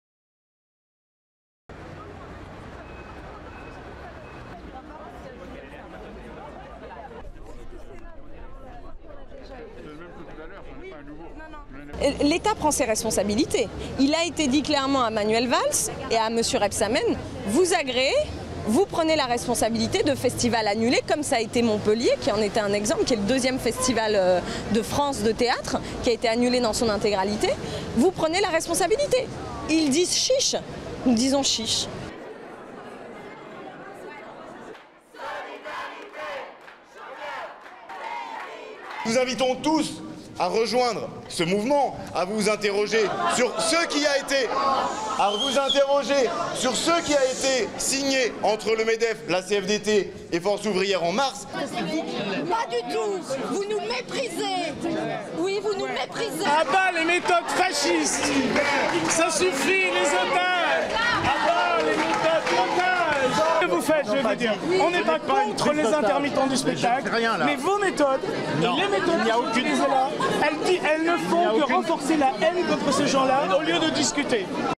Paris : manifestation des intermittents au théâtre de la Ville
Des intermittents se sont invités dimanche au Théâtre de la Ville, à Paris, pour dénoncer l'agrément de la nouvelle convention d'assurance-chômage, à l'occasion d'une représentation de "Palermo Palermo" de Pina Bausch, a indiqué la Coordination des intermittents et précaires d'Ile-de-France (CIP-IDF). Durée:01:15